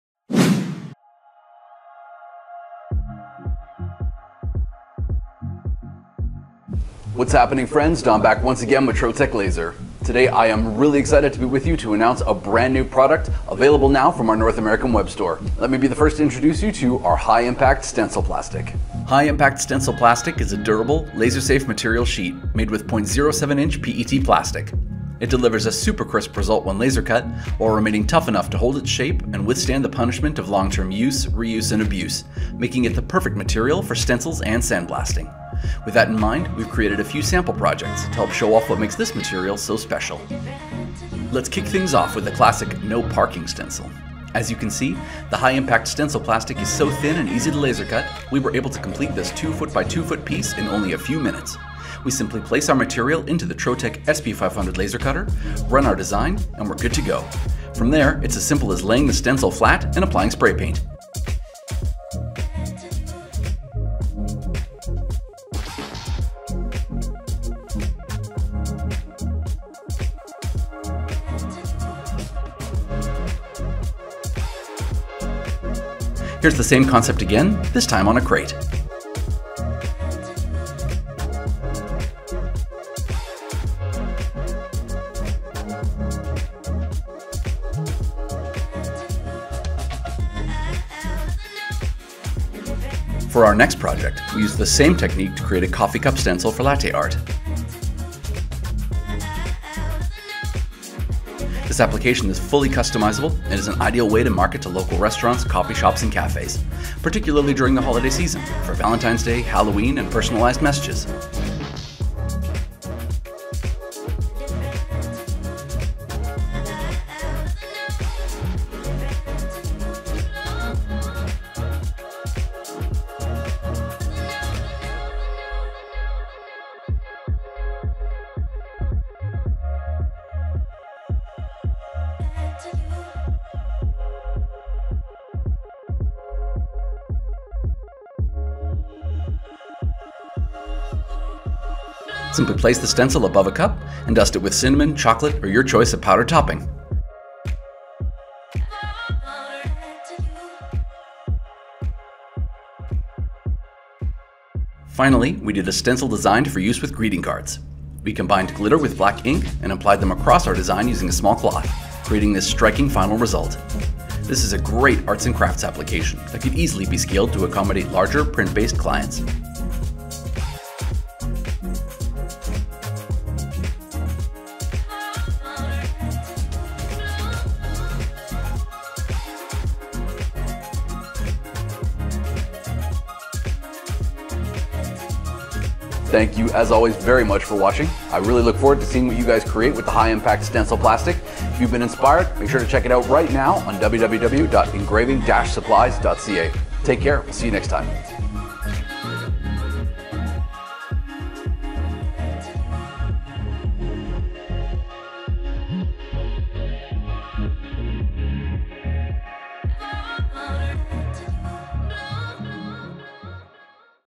Laser Cut Stencils with High Impact Stencil Plastic | Trotec Laser Engraving & Cutting Machines
In this video, we are thrilled to introduce you to our latest product: high-impact stencil plastic, now available for purchase from our North American Web Store. This durable laser-safe material is made from 0.07-inch PET plastic, which delivers a super crisp result when laser cut while remaining tough enough to hold its shape and withstand long-term use, reuse, and abuse. This makes it the perfect material for stencils and sandblasting.

To help showcase the benefits of this new material, we've created several sample projects to demonstrate how easy it is to use and how versatile it can be. For example, we start with the classic "No Parking" stencil, which we were able to complete in just a few minutes using the Trotec SP500 laser cutter. We simply placed the high-impact stencil plastic sheet into the laser cutter, ran our design, and were ready to go. After that, it was as simple as laying the stencil flat and applying spray paint. The resulting stencil was crisp, clean, and durable, making it perfect for outdoor use.

Next, we take the same concept and apply it to a wooden crate, showing how easily the high-impact stencil plastic can be used on a variety of surfaces. Then, we demonstrate how to create a coffee cup stencil for latte art, which is a great way to market to local restaurants, coffee shops, and cafes, particularly during the holiday season. This application is fully customizable and can be used to create personalized messages for Valentine's Day, Halloween, or any other occasion.

Finally, we showcase a stencil design for use with greeting cards. By combining glitter with black ink and applying them across our design using a small cloth, we were able to create a striking final result that is sure to impress. This is a great arts and crafts application that could easily be scaled to accommodate larger print-based clients.

Throughout the video, we demonstrate just how easy it is to work with high-impact stencil plastic, and how it delivers a superior result compared to other stencil materials on the market. We hope that by watching this video, you will be inspired to try high-impact stencil plastic for your next project, whether it's for commercial or personal use. So, if you're looking for a durable, versatile, and high-quality stencil material, be sure to check out high-impact stencil plastic on our website today.